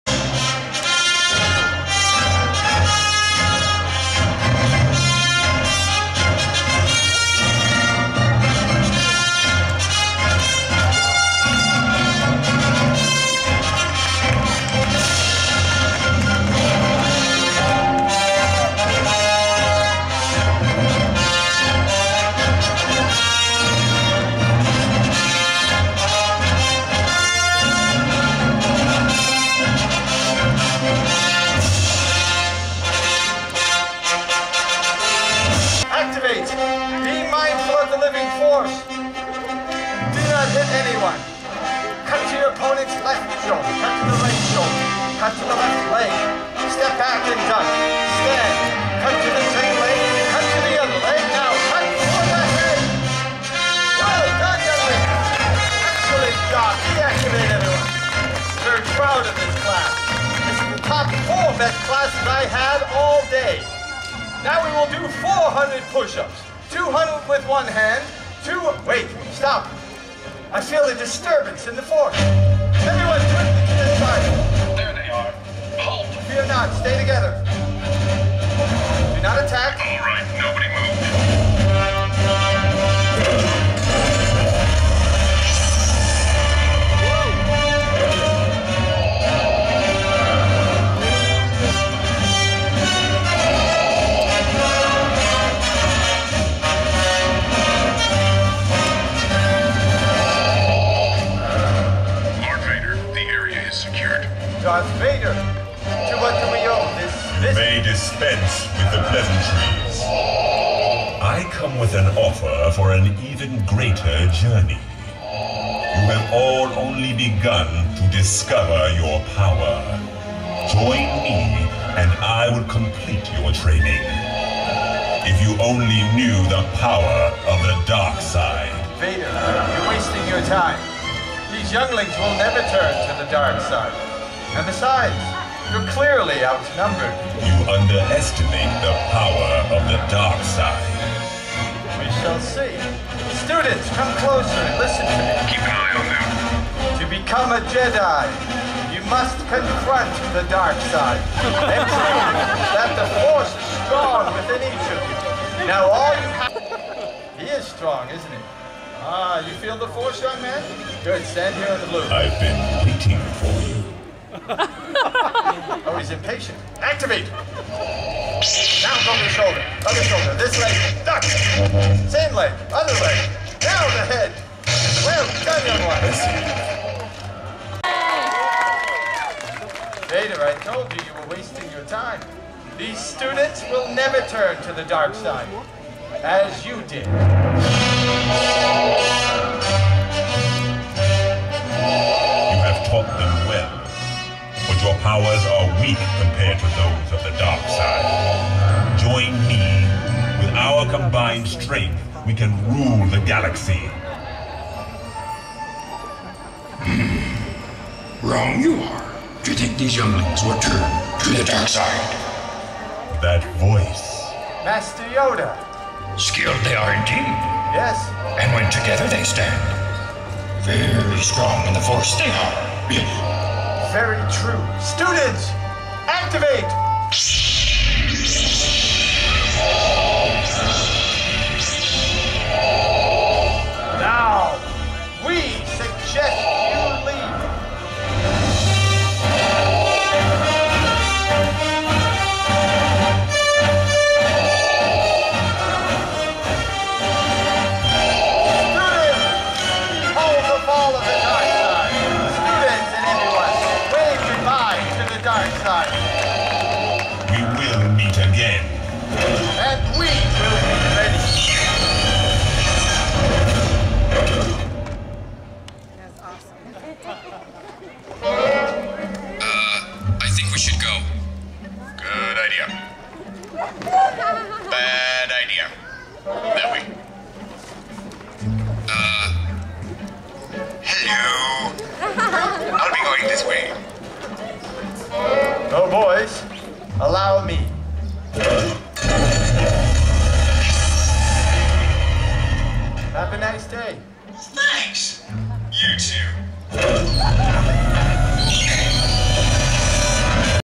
Cordin Schrum Attends Jedi Training Academy
Cordin trains to fight Darth Vader at Disney's Hollywood Studios Jedi Training Academy.  Jan 2010